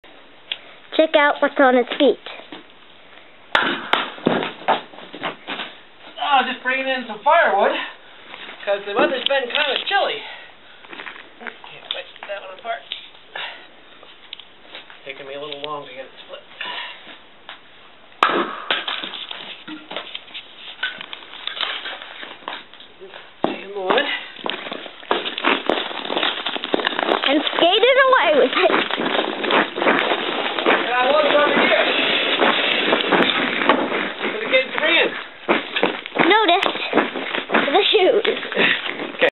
IcyDriveway3.wmv
Carrying in the firewood on an icy driveway.